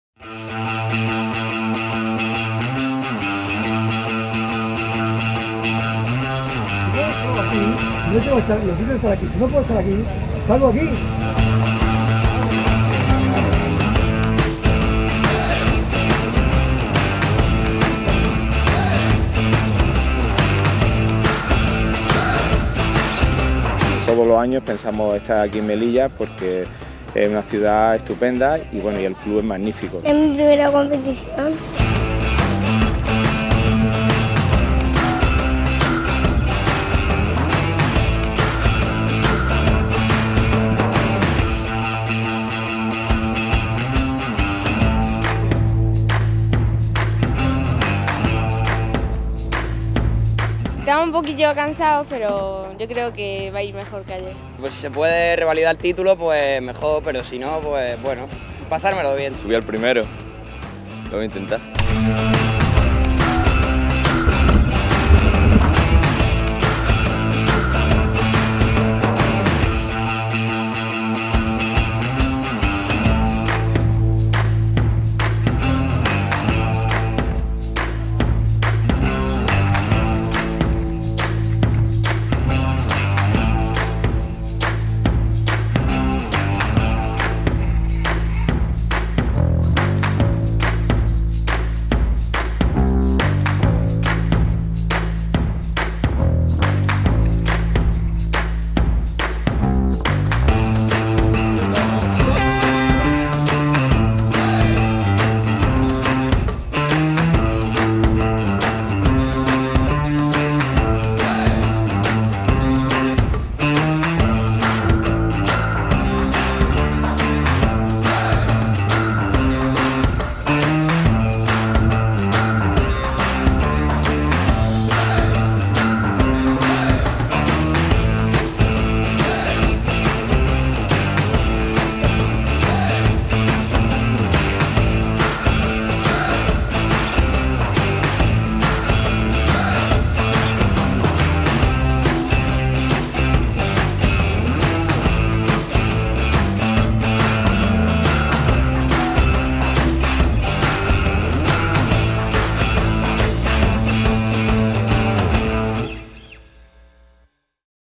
IV Semana Náutica Vela Ligera. 6 sábado
Yolanda Padilla y Antonio Muñoz se proclaman campeones, en Optimist y Láser 4.7 respectivamente, de la IV Semana Náutica de Vela Ligera de la ciudad de Melilla. A continuación un resumen de lo que estos chicos nos han hecho disfrutar.